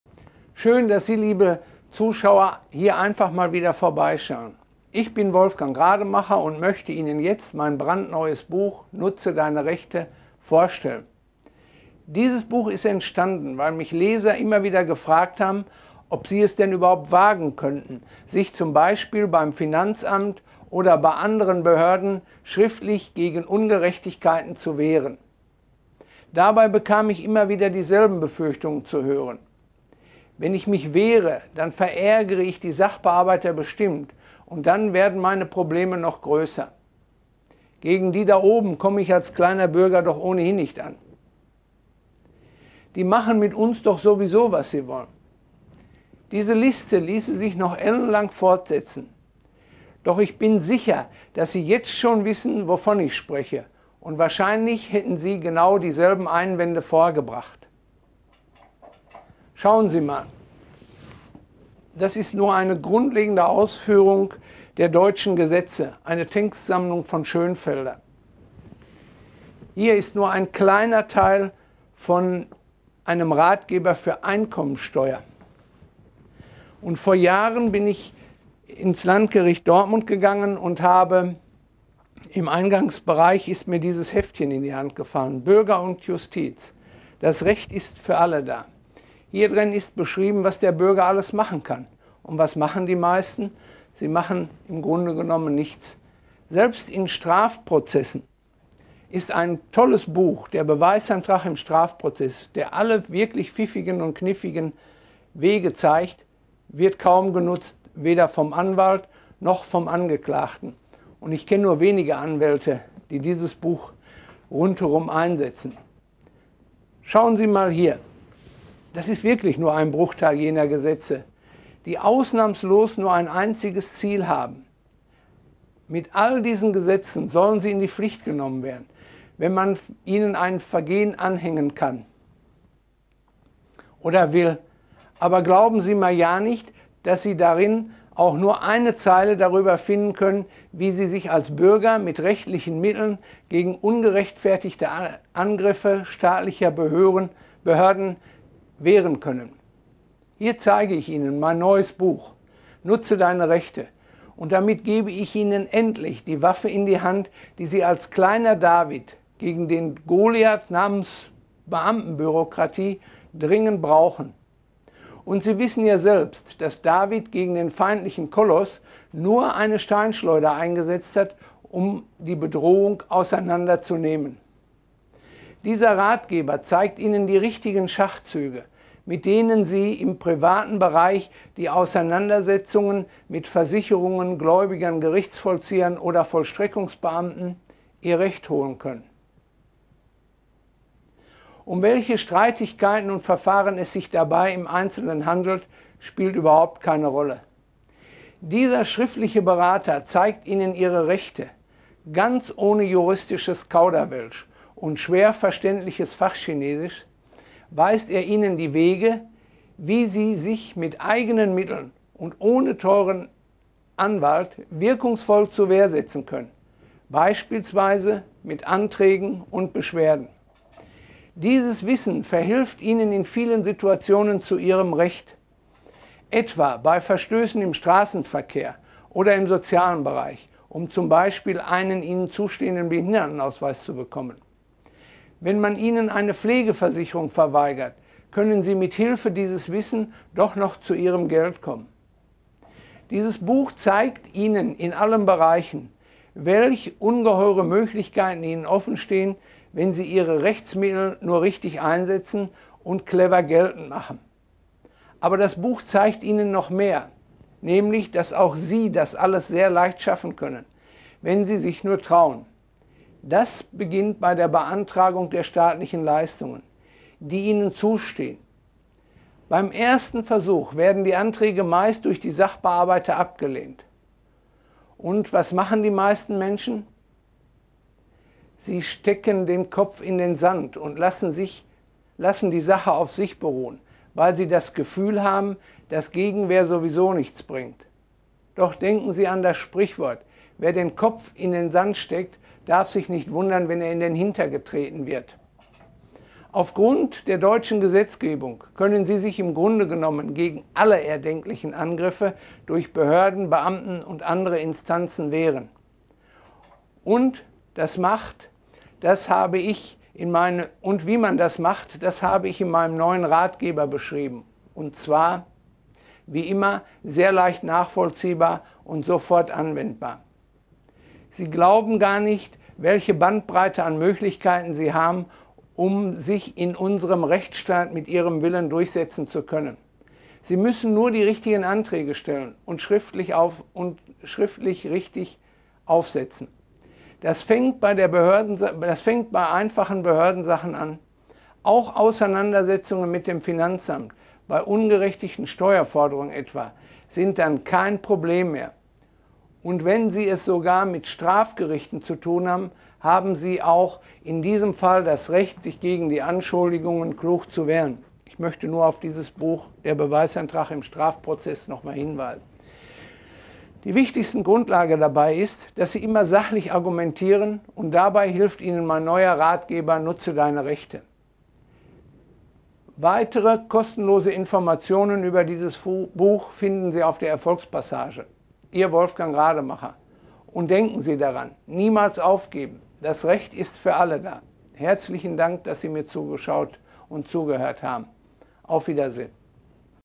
Nutze Deine Rechte - Das Recht ist für alle da - Achtung Videoinfos lesen
Schön, dass Sie liebe Zuschauer, hier einfach mal vorbeischauen. Ich bin Wolfgang Rademacher und möchte Ihnen jetzt mein brandneues Buch »Nutze Deine Rechte« vorstellen.
Dieses Buch ist entstanden, weil mich Leser immer wieder gefragt haben, ob sie es denn überhaupt wagen könnten, sich zum Beispiel beim Finanzamt oder bei anderen Behörden schriftlich gegen Ungerechtigkeiten zu wehren.
Dabei bekam ich immer wieder dieselben Befürchtungen zu hören:
• Wenn ich mich wehre, dann verärgere ich die Sachbearbeiter bestimmt, und dann werden meine Probleme noch größer!
• Gegen die da oben komme ich als kleiner Bürger doch ohnehin nicht an!
• Die machen mit uns doch sowieso, was sie wollen!
Diese Liste ließe sich noch ellenlang fortsetzen. Doch ich bin sicher, dass Sie jetzt schon wissen, wovon ich spreche. Und wahrscheinlich hätten Sie genau dieselben Einwände vorgebracht.
Hier zeige ich Ihnen das Bürgerliche Gesetzbuch.
Schauen Sie mal hier ... das ist nur ein Bruchteil jener Gesetzestexte, die ausnahmslos nur ein einziges Ziel haben: Mit all diesen Gesetzen sollen Sie in die Pflicht genommen werden, wenn man Ihnen ein Vergehen anhängen will. Aber glauben Sie mal ja nicht, dass Sie darin auch nur eine Zeile darüber finden können, wie Sie sich als Bürger mit rechtlichen Mitteln gegen ungerechtfertigte Angriffe staatlicher Behörden wehren können.
Hier zeige ich mein neues Buch »Nutze Deine Rechte«, und damit gebe ich Ihnen endlich DIE Waffe in die Hand, die Sie als kleiner David gegen den Goliath namens Beamten-Bürokratie dringend brauchen. Und Sie wissen ja selbst, dass David gegen den feindlichen Koloss nur eine Steinschleuder eingesetzt hat, um die Bedrohung auseinander zu nehmen.
Dieser Ratgeber zeigt Ihnen die richtigen Schachzüge, mit denen Sie sich im privaten Bereich bei Auseinandersetzungen mit Versicherungen, Gläubigern, Gerichtsvollziehern oder Vollstreckungsbeamten Ihr Recht holen können. Um welche Streitigkeiten und  Verfahren es sich dabei im Einzelnen handelt, spielt überhaupt keine Rolle.
Dieser schriftliche Berater zeigt Ihnen Ihre Rechte. Ganz ohne juristisches Kauderwelsch oder schwer verständliches Fach-Chinesisch weist er Ihnen die Wege, wie Sie sich mit eigenen Mitteln und ohne teuren Anwalt wirkungsvoll zur Wehr setzen können -- beispielsweise mit Anträgen und Beschwerden.
Dieses Wissen verhilft Ihnen in vielen Situationen zu Ihrem Recht, etwa bei Verstößen im Straßenverkehr oder im sozialen Bereich, um z.B. einen Ihnen zustehenden Behindertenausweis zu bekommen. Wenn man Ihnen eine Pflegeversicherung verweigert, könnten Sie mit Hilfe dieses Wissens doch noch zu Ihrem Geld kommen.
Dieses Buch zeigt Ihnen in allen Bereichen, welch ungeheure Möglichkeit Ihnen offenstehen, wenn Sie ihre Rechtsmittel nur richtig einsetzen und clever geltend machen. Aber das Buch zeigt Ihnen noch mehr -- nämlich, dass auch Sie das alles sehr leicht schaffen können, wenn Sie sich nur trauen.
Das beginnt bei der Beantragung der staatlichen Leistungen, die Ihnen zustehen. Beim ersten Versuch werden die Anträge meistens durch die Sachbearbeiter abgelehnt. Und was machen die meisten Menschen? Sie stecken den Kopf in den Sand und lassen die Sache auf sich beruhen, weil Sie das Gefühl haben, dass Gegenwehr sowieso nichts bringt. Doch denken Sie an das Sprichwort: Wer den Kopf in den Sand steckt, darf sich nicht wundern, wenn er in den Hintern getreten wird.
Aufgrund der deutschen Gesetzgebung können Sie sich im Grunde genommen gegen alle erdenklichen Angriffe durch Behörden, Beamte und andere Instanzen wehren. Wie man das macht, das habe ich in meinem neuen Ratgeber beschrieben. Und zwar wie immer sehr leicht nachvollziehbar und sofort anwendbar.
Sie glauben gar nicht, welche Bandbreite an Möglichkeiten Sie haben, um sich in unserem Rechtsstaat mit Ihrem Willen durchsetzen zu können. Sie müssen nur die richtigen Anträge stellen und Schriftstücke richtig aufsetzen.
Das fängt bei einfachen Behördensachen an. Auch Auseinandersetzungen mit dem Finanzamt -- bei ungerechtfertigten Steuerforderungen etwa -- sind dann kein Problem mehr. Und wenn Sie es sogar mit Strafgerichten zu tun haben, haben Sie auch in diesem Fall das Recht, sich gegen die Anschuldigungen klug zu wehren.
Die wichtigste Grundlage dabei ist, dass Sie immer sachlich argumentieren. Und dabei hilft Ihnen mein neuer Ratgeber »Nutze Deine Rechte«.

Ihr Wolfgang Rademacher

Und denken Sie daran: »Niemals Aufgeben« - Das Recht ist für alle da!